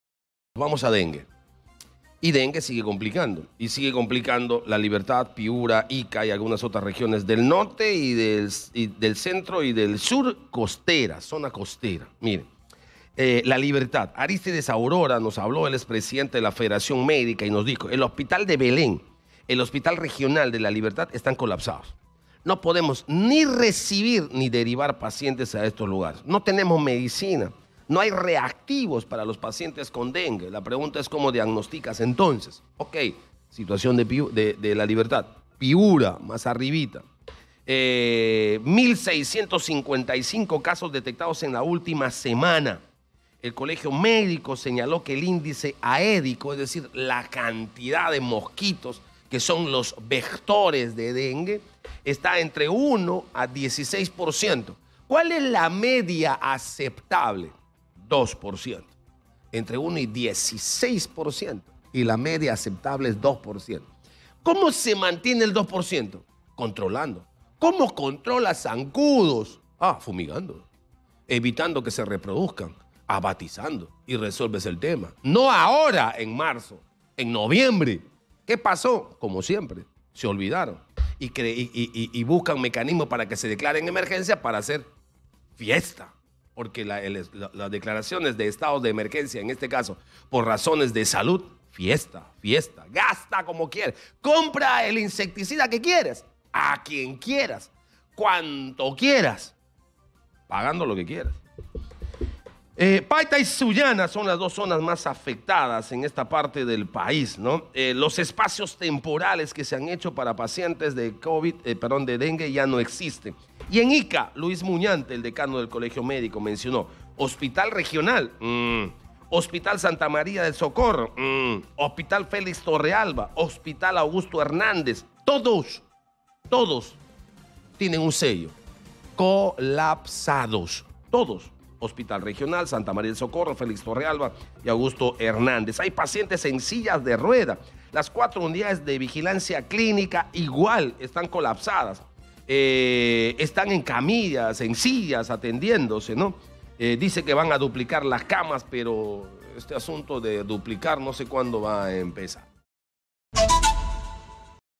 🔴🔵Hospitales de Ica, Lambayeque y Piura colapsan ante aumento de casos de dengue
La epidemia del dengue en el Perú ha provocado que continúe el aumento el número de pacientes y fallecidos producto de la enfermedad tropical. El preocupante escenario ha llevado a que los hospitales de Piura, La Libertad e Ica colapsen y ya no puedan atender a más pacientes.

Exitosa Perú
"Exitosa: La voz que integra al Perú"

Corporación Universal